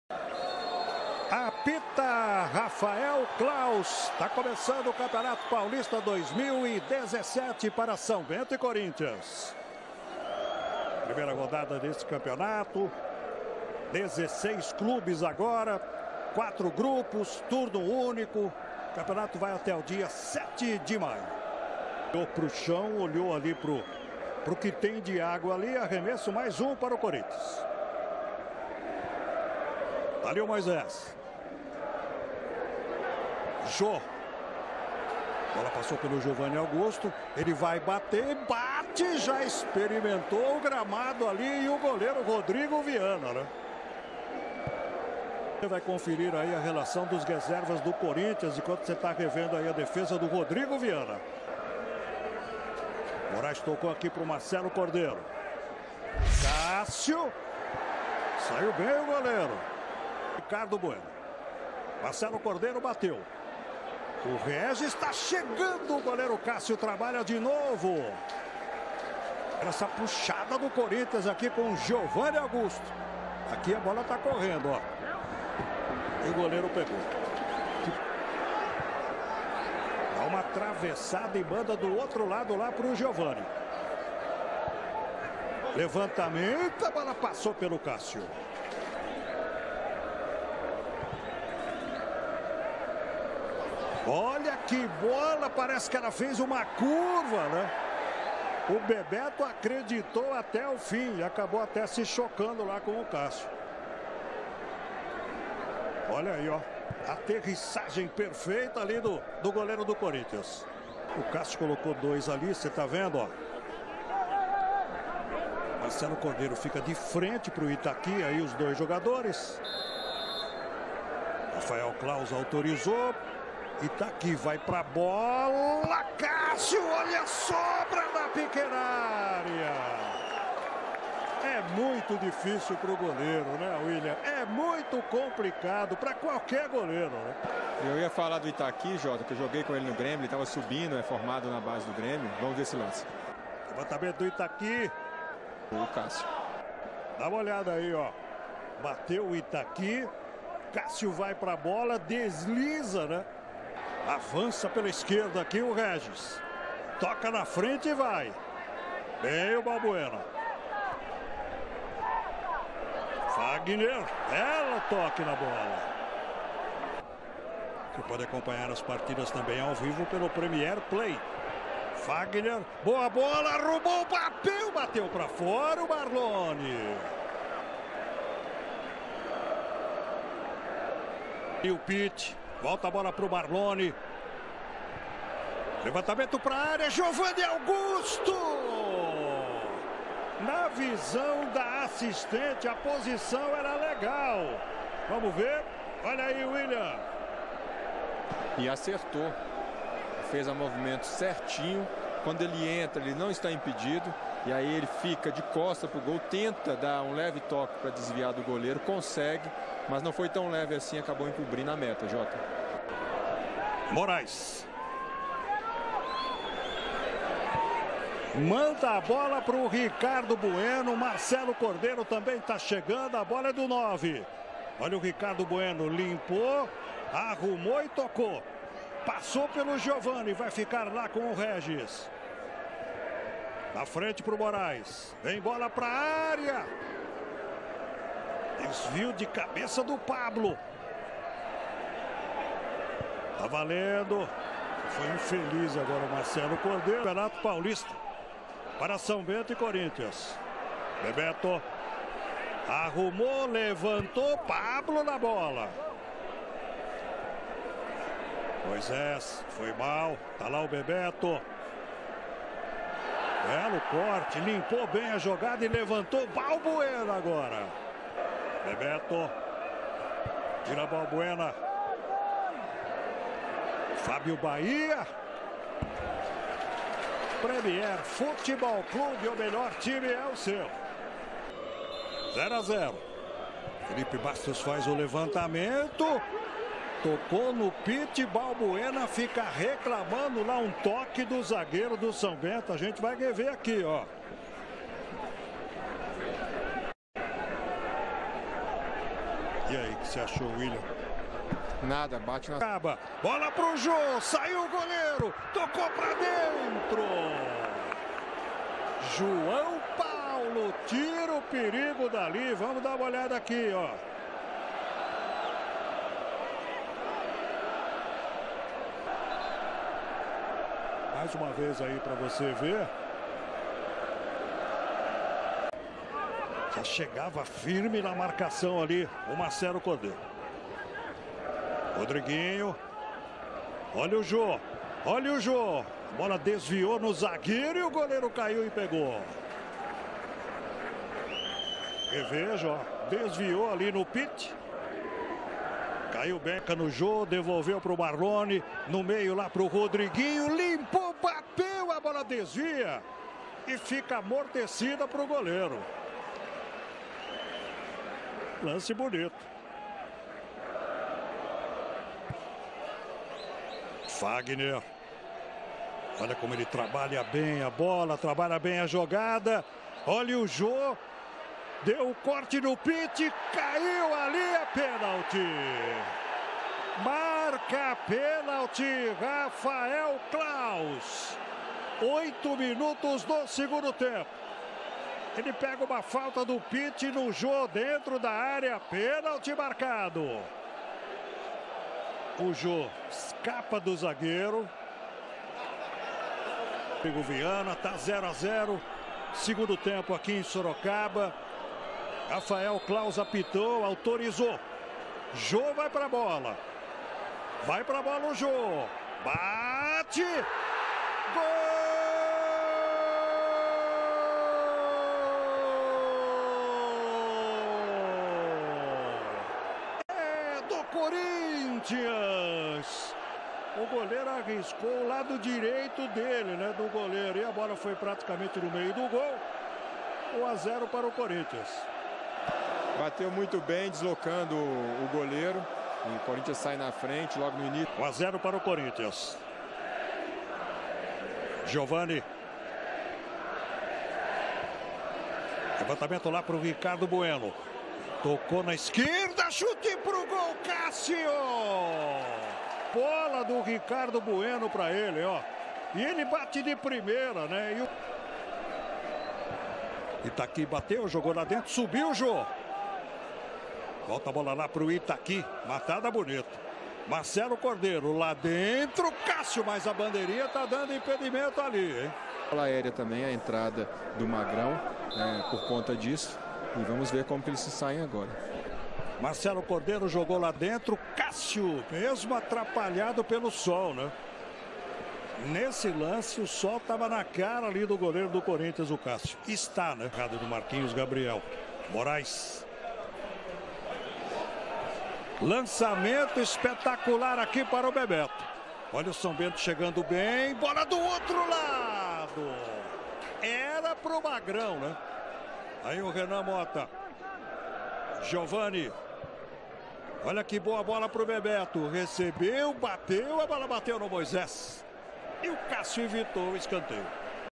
sbento 0x1 CORINTHIANS 04/2/2017 1ª ROD PAULISTA
s.bento 0 X 1 CORINTHIANS
Paulistao 1ª rodada
Local: Estádio Walter Ribeiro- sorocaba
Data-Hora: 4/2/2017- sabado - 17h
Arb: Raphael Claus, Tatiane Sacilotti e Evandro de Melo Lima
Público/renda: 6.007 presentes/ R$ 362.705,00
Cartões amarelos: Pitty (SBE), Gabriel, Balbuena (COR)
Cartões vermelhos: -
Gol: Jô (9'/2ºT) (0-1) penalti

sbento: 1-Rodrigo Viana; 2-Régis Souza (16-Renan Mota, aos 21'/2T), 4-João Paulo, 3-Pitty e 6-Marcelo Cordeiro; 7-Bebeto, 5-Fabio Bahia, 11-Itaqui, 8-Morais e 10-Giovanni (18-Clebson, aos 30/2T); 9-Ricardo Bueno (20-Magrão, aos 16'/2T). Técnico: Paulo Roberto. + 42-Andrey, 13-Lucas Mendes, 14-Gabriel, 15-Moradei, 17-Guilherme Queiroz, 19-Jobinho

CORINTHIANS: 12-Cássio; 23-Fagner, 4-Balbuena, 3-Pablo e 6-Moisés; 5-Gabriel; 21-Fellipe Bastos (29-Camacho, aos 30'/2T), 17-Giovanni Augusto (31-Marquinhos Gabriel, aos 21'/2T), 26-Rodriguinho e 8-Marlone; 7-Jô (11-Romero, aos 34'/2T). Técnico: Fabio Carille. + 1-Caíque França, 32-Léo Principe, 14-Pedro Henrique, 15-Vilson, 28-Paulo Roberto, 22-Marciel, 10-Guilherme